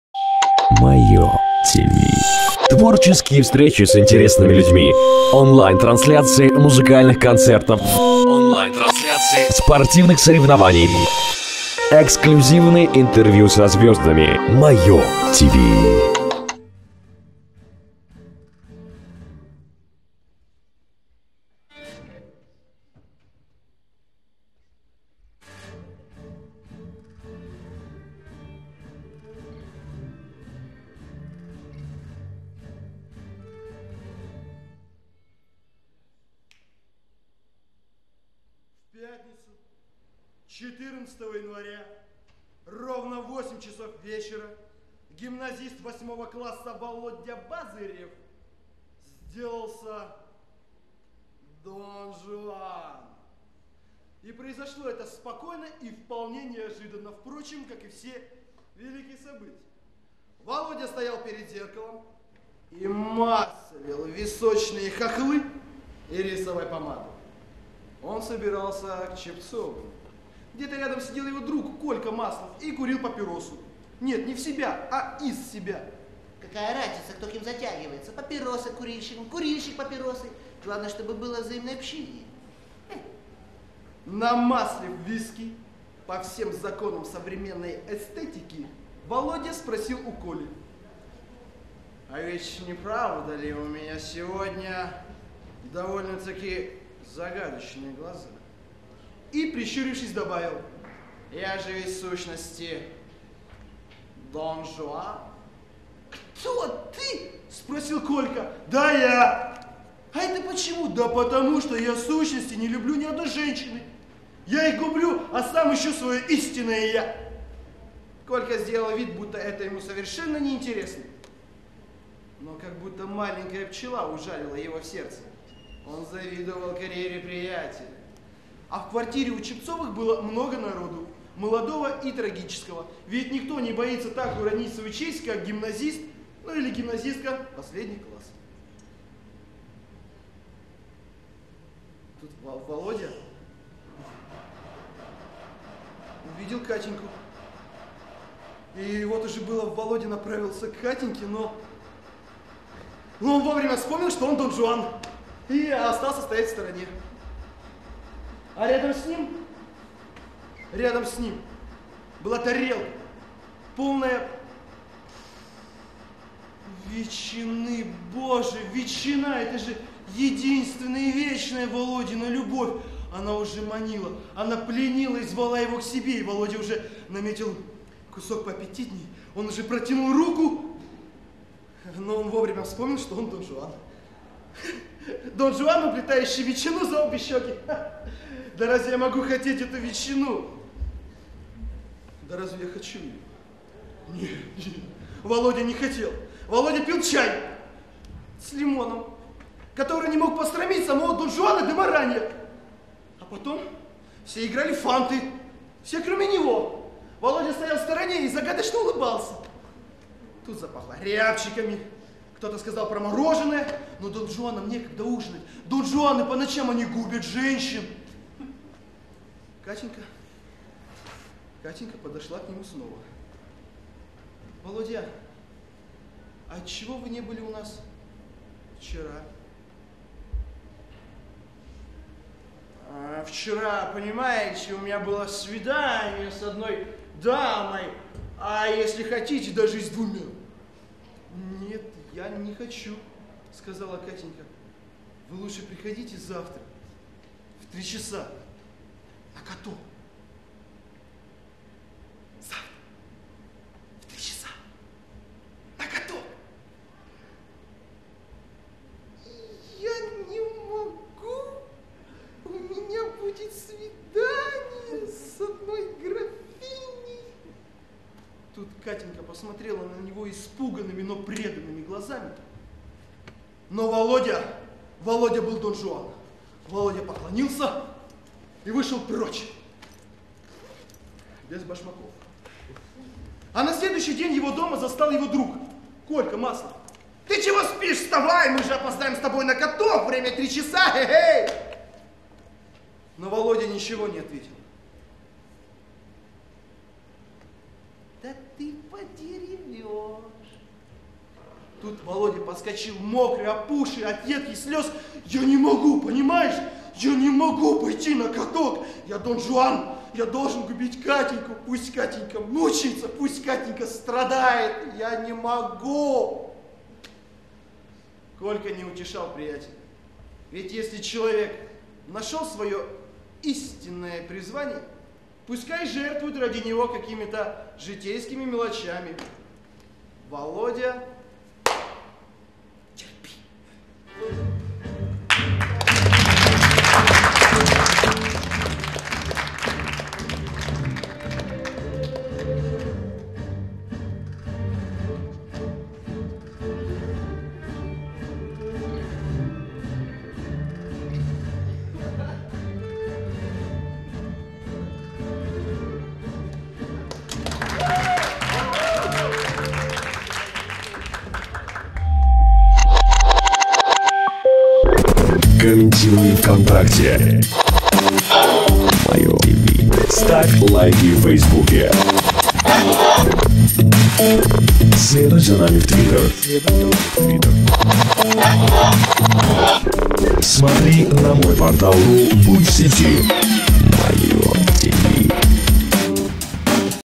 Антон Лаврёнов "Дон Жуан" | Студвесна - ТЕАТР
Номинация "Эстрадный монолог"
Антон Лаврёнов "Дон Жуан"
Тюменская государственная академия культуры, искусств и социальных технологий
Смотри Тюменскую областную студенческую весну 2013 на